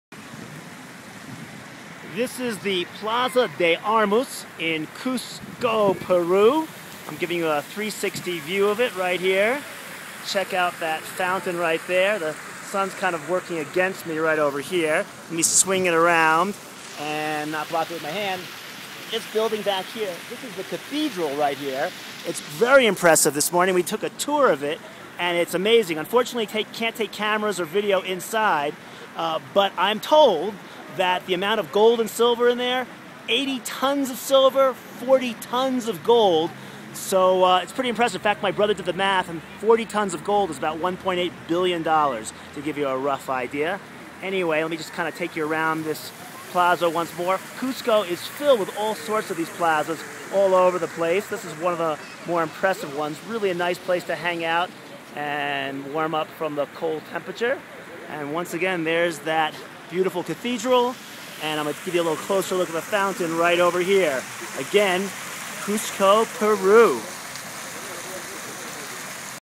Cuzco Peru Plaza
A 360 degree tour of one of the many picturesque plazas in Cuzco Peru